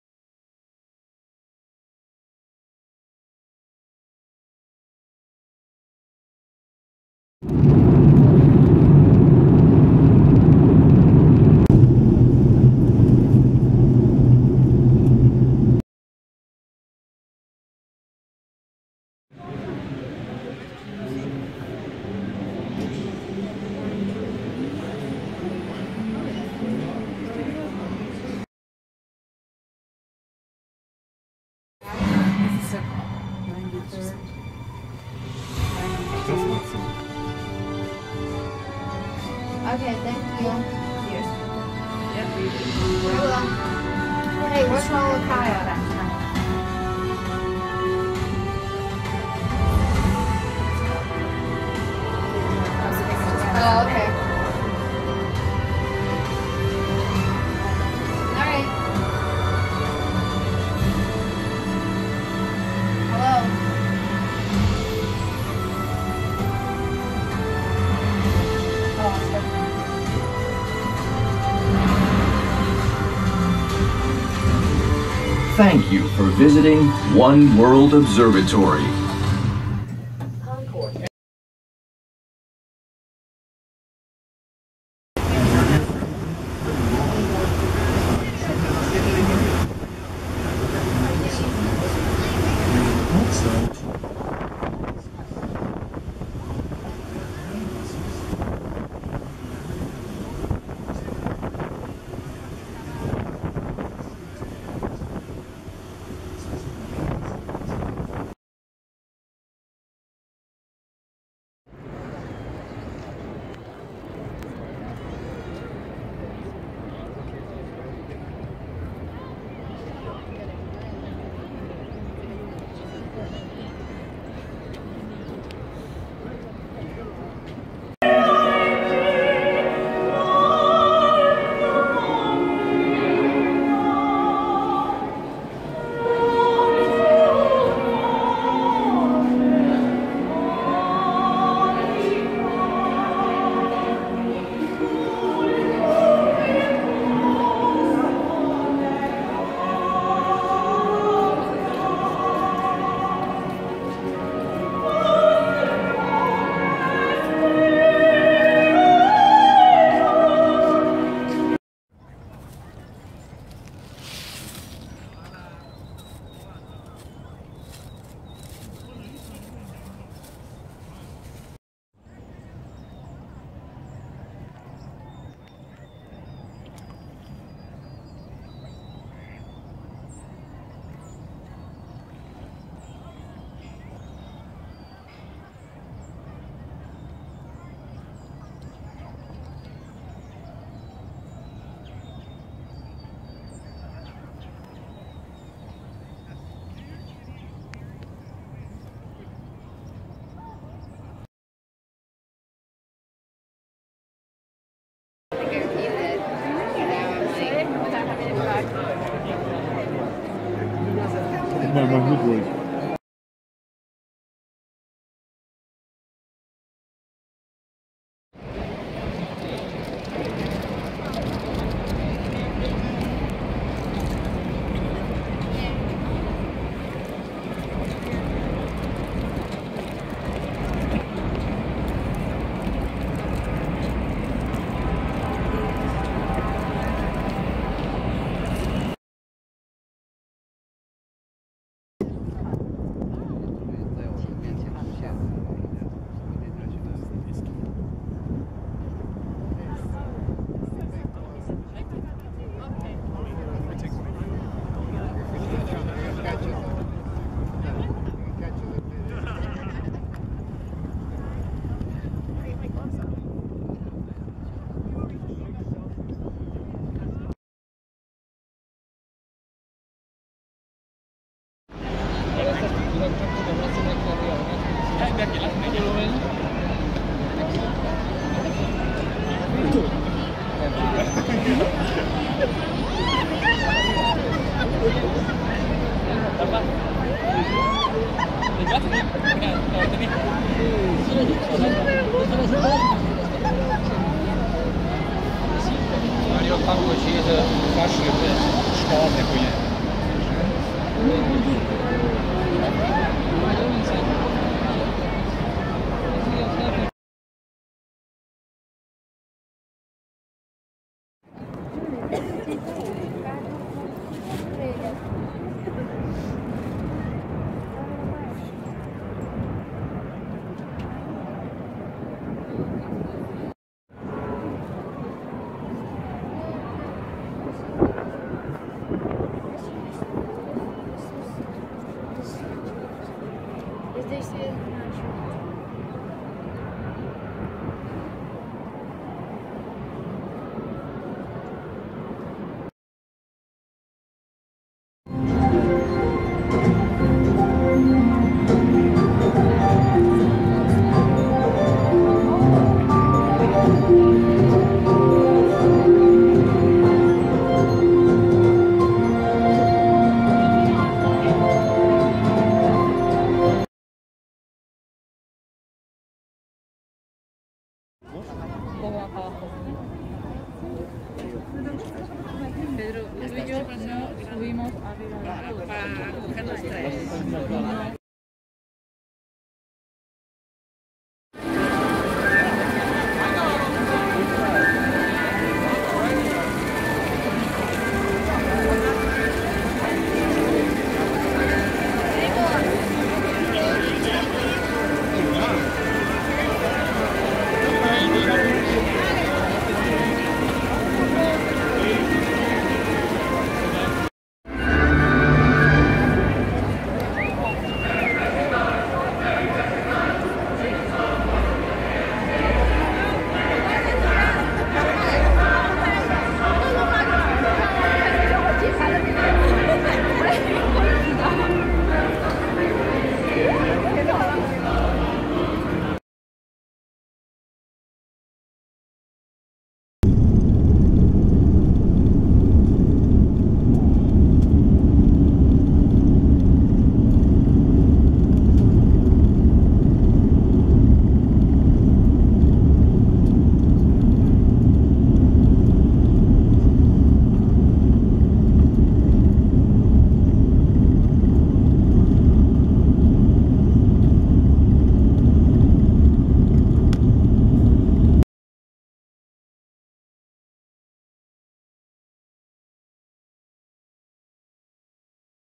New York in der Vorweihnachtszeit
Kurzes Video von ein paar Impressionen aus New York im Dezember 2016.
Ca am 16. online gehen.